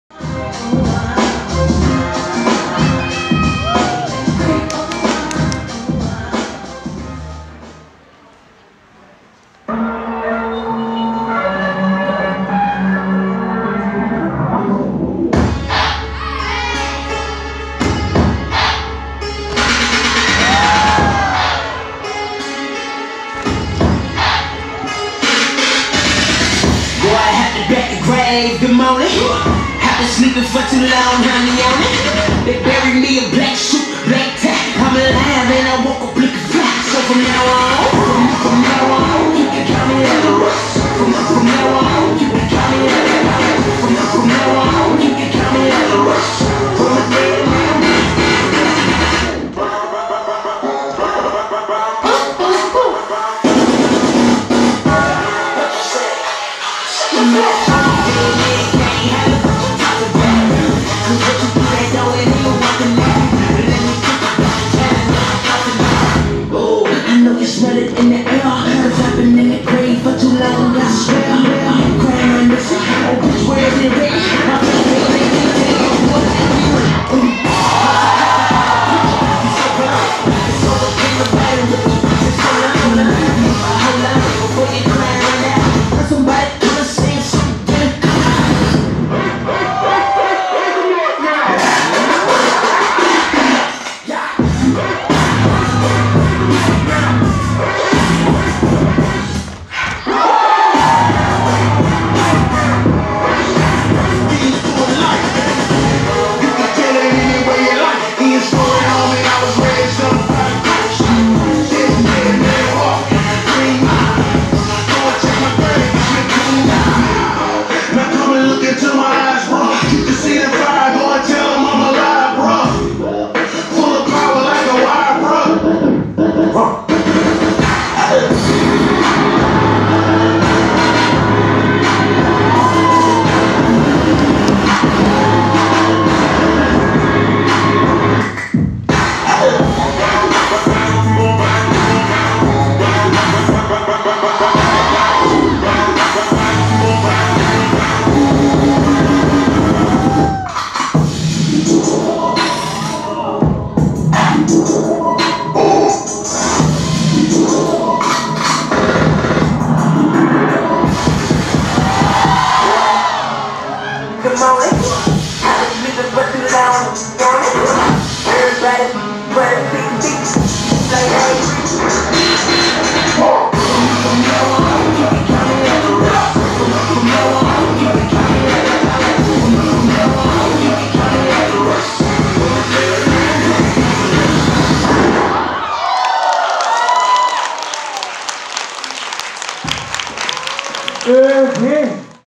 NOLIP | 2023 P.TYPE | 피타입 퍼포먼스 대회
2023 P.TYPE VOL.1
ALLROUND PERFORMANCE COMPETITION

*JUDGE
-RIHEY
-WOOTAE
-RAN

*MC
KYAM

*DJ
SOM

*FILM
YEN

*SHOWCASE
OHBODY&WOOTAE (WILDYMOTION)